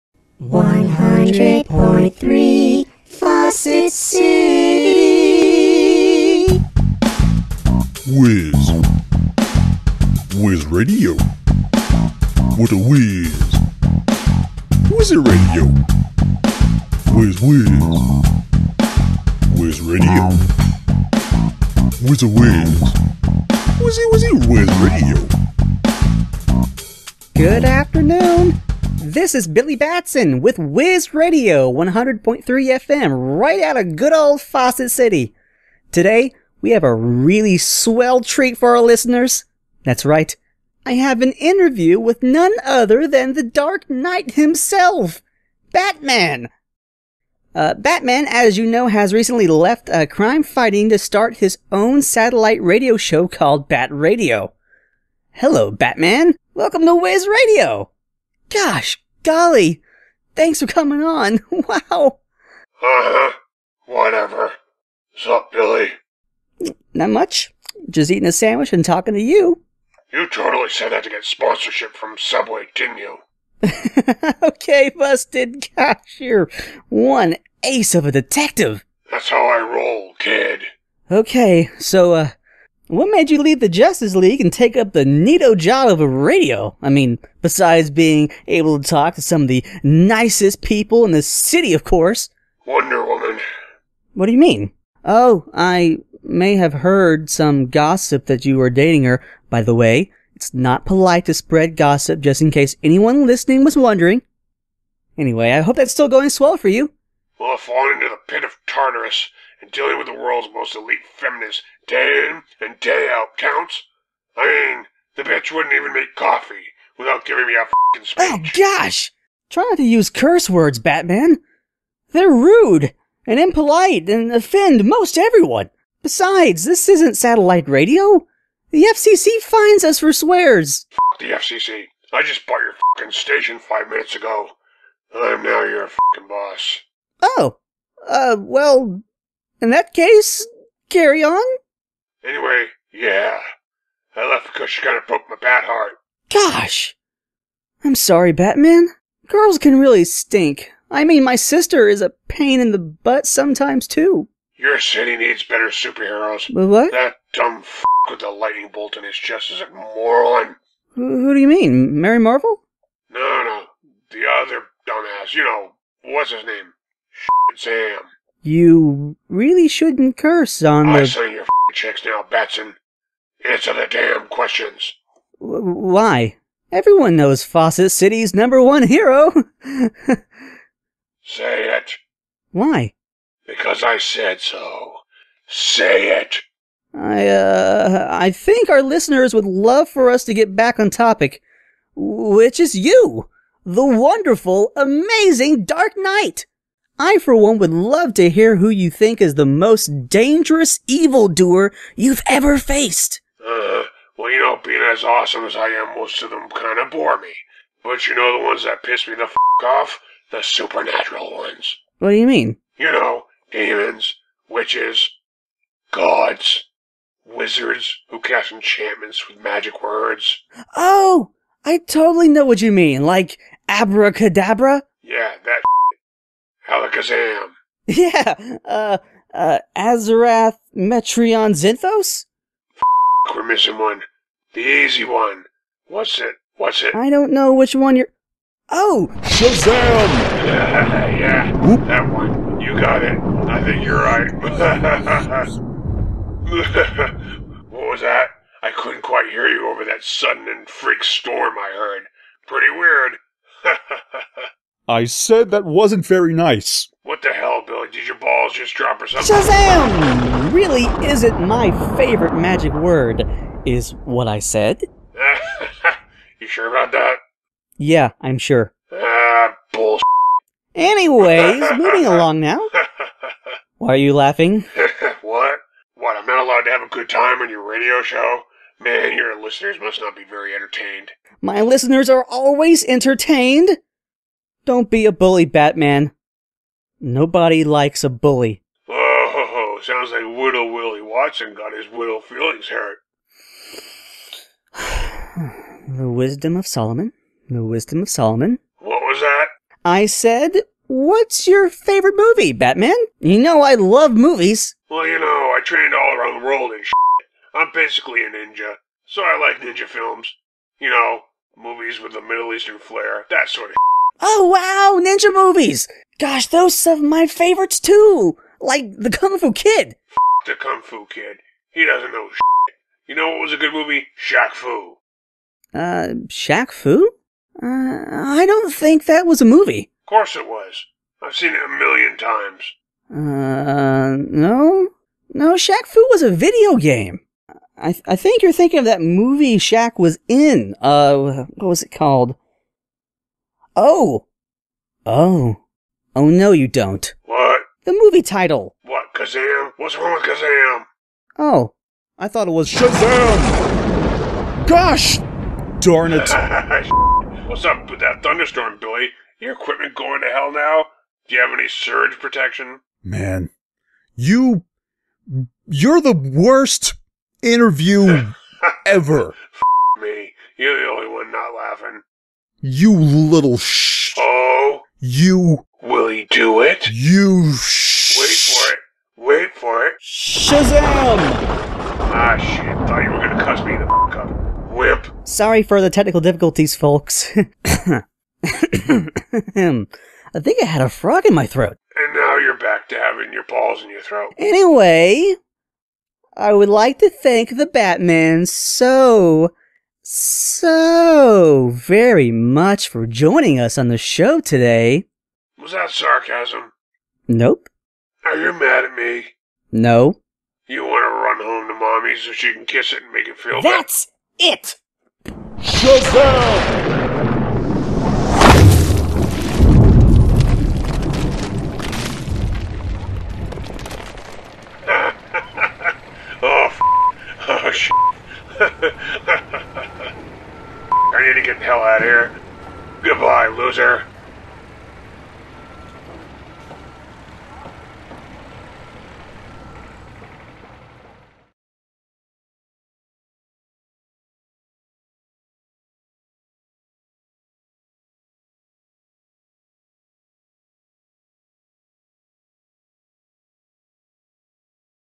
Bat Radio Minisode Say the Word Censored version
When Batman is interviewed by Whiz radio's Billy Batson, Batman decided to troll him into saying his magic word.

Starring George Silsby as Batman, Heath Rowell as Billy Batson/Shazam

Edited by Heath Rowell

Co written by Heath Rowell and George Silsby